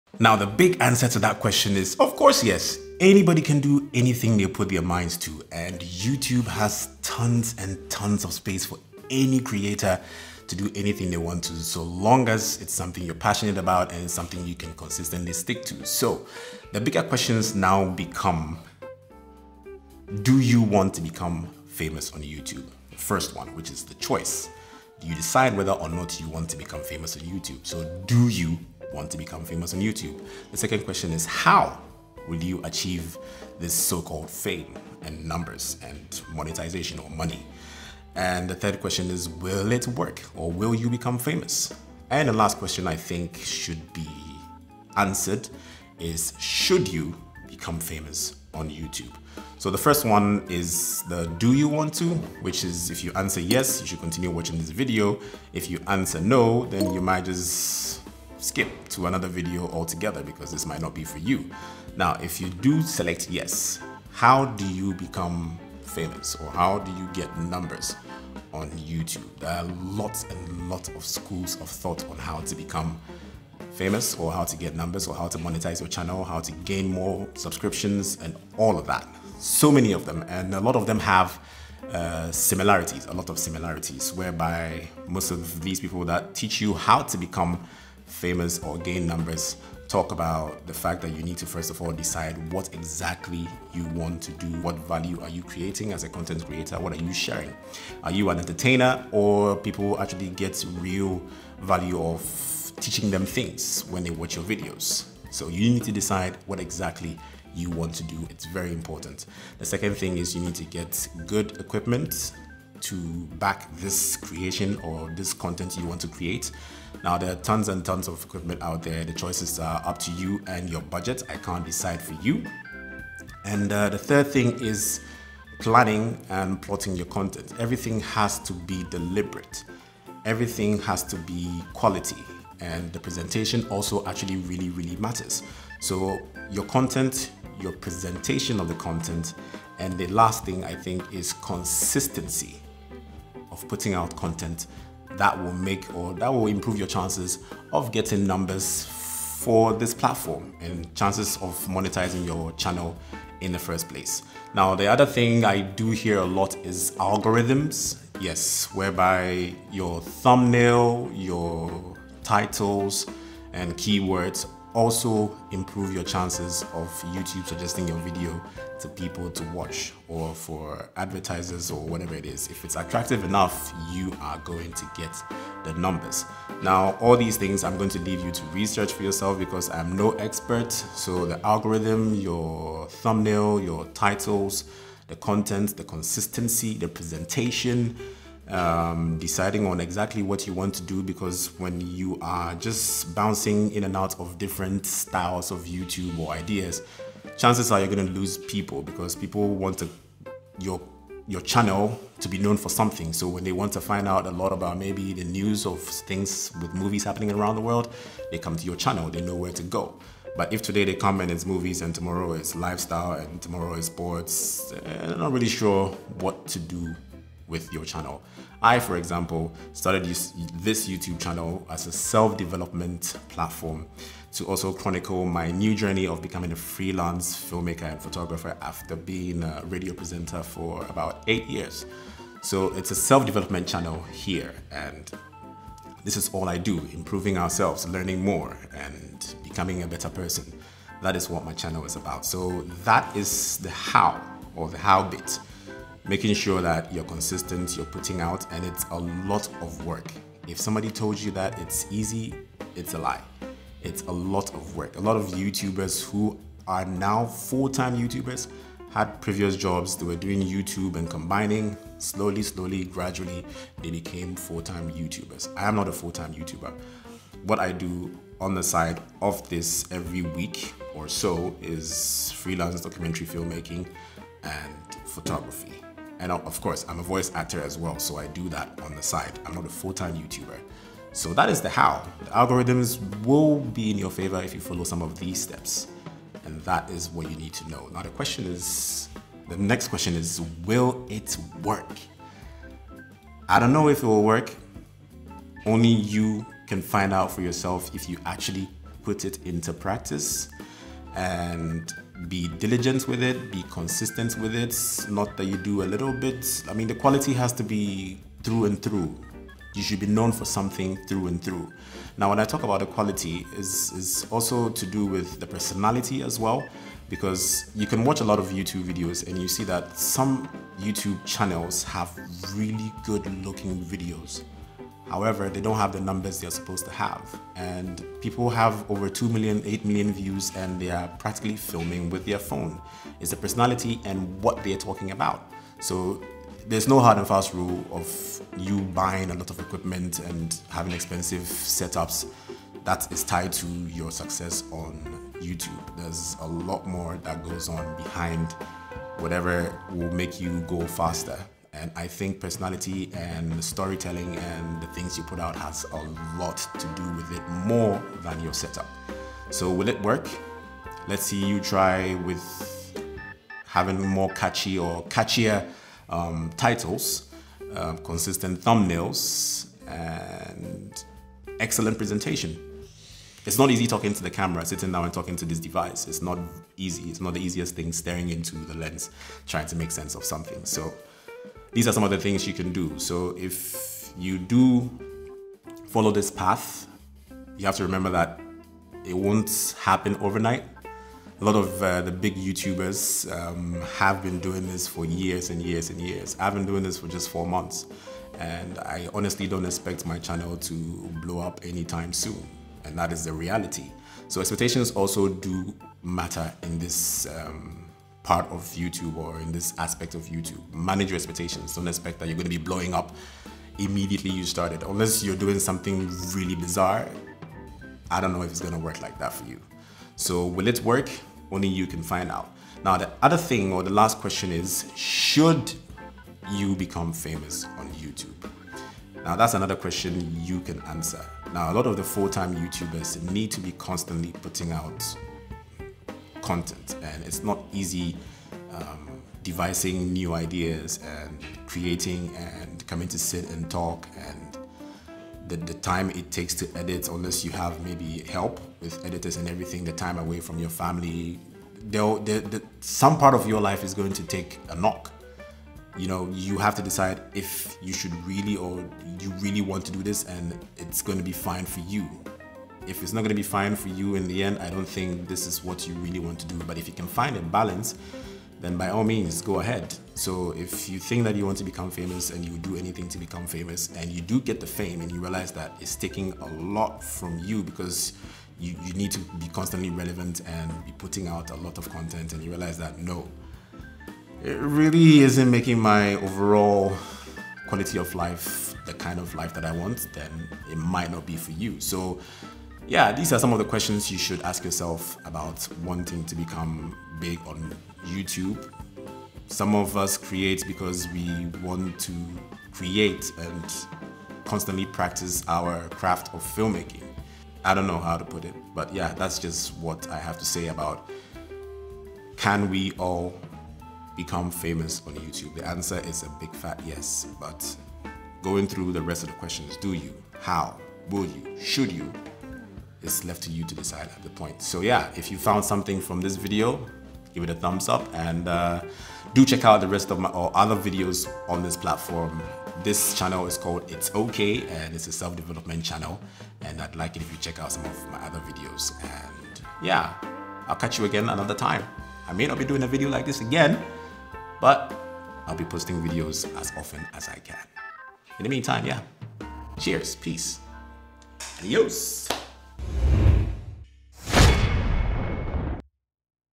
Can we all make it BIG on Youtube? Y/N?
Do you want to get more views and increase your subscription numbers? Well me too!

This video is exploring all the tips and tricks to get more views, get more subscribers and earn more on youtube with your content.

These are a few tips summarised from experts on how to make it big on Youtube.

This video in itself is an experiment to see IF indeed these theories work.